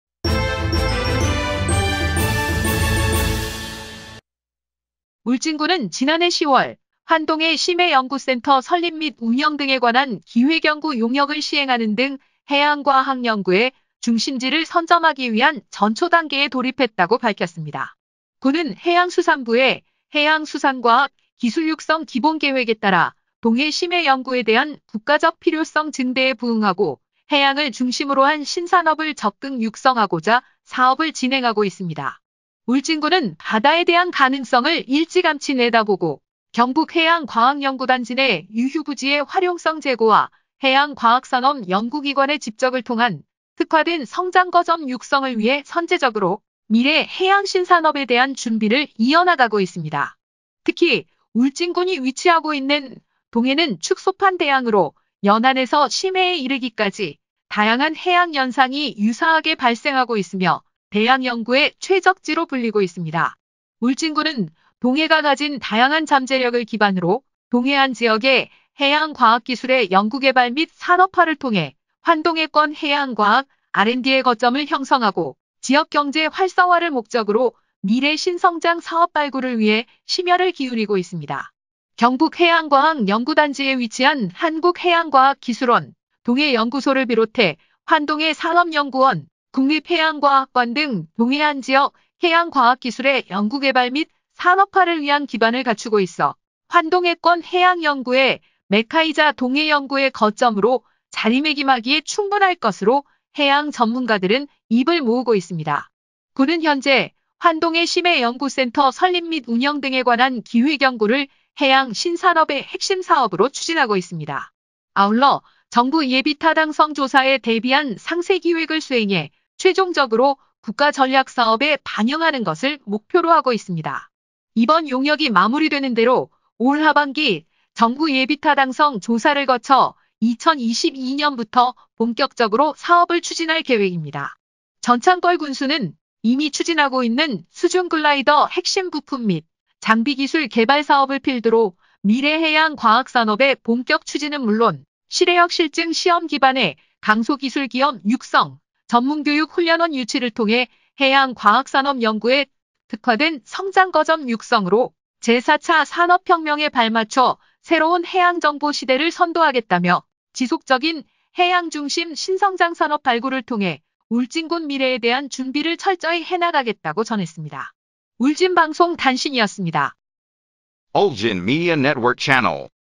[울진방송] 20210112 울진군, 미래 해양신산업 청사진 준비 ‘착착’
김영규 PD

미래 해양신산업에 대한 선제적 준비

울진군(군수 전찬걸)은 지난 해 10월 ‘환동해 심해연구센터 설립 및 운영 등에 관한
기획연구 용역’을 시행하는 등 해양과학연구의 중심지를 선점하기 위한 전초단계에
돌입했다고 밝혔습니다.
군은 해양수산부의 해양수산과학기술육성 기본계획에 따라 동해 심해 연구에 대한 국가적
필요성 증대에 부응하고, 해양을 중심으로 한 신산업을 적극 육성하고자 사업을 진행하고
있습니다.
해양은 생물자원과 광물자원, 수자원, 에너지자원의 보고로써 오늘날 인류가 직면한
육상자원의 고갈, 환경오염 및  기후변화 등의 문제를 해결하고 미래국가발전을
견인할 수 있는 필요충분조건을 완비하고 있어, 인류가 직면한 위기의 대안지로 국제사회에서
평가받고 있습니다.
울진군은 바다에 대한 가능성을 일찌감치 내다보고, 경북해양과학연구단지(GMSP) 내
유휴부지의 활용성 제고와 해양과학산업 연구기관의 집적을 통한 특화된 성장거점
육성을 위해 선제적으로 미래 해양신산업에 대한 준비를 이어나가고 있습니다.
특히, 울진군이 위치하고 있는 동해는 ‘축소판 대양’으로 연안에서 심해에 이르기까지
다양한 해양현상이 유사하게 발생하고 있으며, 대양연구의 최적지로 불리고 있습니다.

동해에서는 기후 및 환경변화, 지질구조, 생태계 등 다양한 해양연구가 가능하며,
가스 하이드레이트 등 고부가가치 해양자원이 풍부하고, 난․한류가 교차하여
다양한 생물자원과 바람, 파도, 태양광 등 해양에너지원의 자연적 조건이 우수합니다.

울진군은 동해가 가진 다양한 잠재력을 기반으로 동해안 지역의 해양과학기술의 연구개발 및
산업화를 통해 환동해권 해양과학 R&D의 거점을 형성하고 지역경제의 활성화를 목적으로
미래 신성장 사업 발굴을 위해 심혈을 기울이고 있습니다.

경북해양과학연구단지(GMSP)에 위치한 한국해양과학기술원 동해연구소를 비롯해,
환동해산업연구원, 국립해양과학관 등 동해안 지역의 해양과학기술의 연구개발 및
산업화를 위한 기반을 갖추고 있어 환동해권 해양연구의 메카이자 동해연구의 거점으로
자리매김하기에 충분할 것으로 해양 전문가들은 입을 모으고 있습니다.

군은 현재 ‘환동해 심해연구센터 설립 및 운영 등에 관한 기획연구’를 해양신산업의
핵심사업으로 추진하고 있습니다.

이 용역의 주요 과업내용은
▲센터설립의 필요성과 타당성 분석, 기본 구상안 및 기본계획 수립
▲건립 후 지속적인 운영을 위한 예산 확보 및 운영방안 마련
▲현재 추진 및 계획중인 해양과학 연구분야 사업과의 연계방안 및 통합적 검토
▲해양연구기관(동해연구소, 환동해산업연구원, 국립해양과학관)과의 상생발전 방안
모색 등의 내용이 포함되어 있습니다.

아울러, 정부 예비타당성조사에 대비한 상세기획을 수행해 최종적으로 국가 전략사업에 반영하는 것을 목표로 하고 있습니다.
이번 용역이 마무리 되는 대로 올 하반기 정부 예비타당성 조사를 거쳐 2022년부터 본격적으로 사업을 추진할 계획입니다.

‘이미 추진하고 있는 수중글라이더 핵심부품 및 장비기술 개발사업을 필두로 미래 해양과학산업의 본격 추진은 물론, 실해역 실증․시험기반의 강소기술기업 육성, 전문교육·훈련원 유치를 통해
해양과학산업 연구의 특화된 성장거점 육성으로 제4차 산업혁명에 발맞춰 새로운 해양정보
시대를 선도하겠다’며 ‘지속적인 해양중심 신성장산업 발굴을 통해 울진군 미래에 대한 준비를
철저히 해나가겠다’고 밝혔습니다.

울진방송 뉴스였습니다.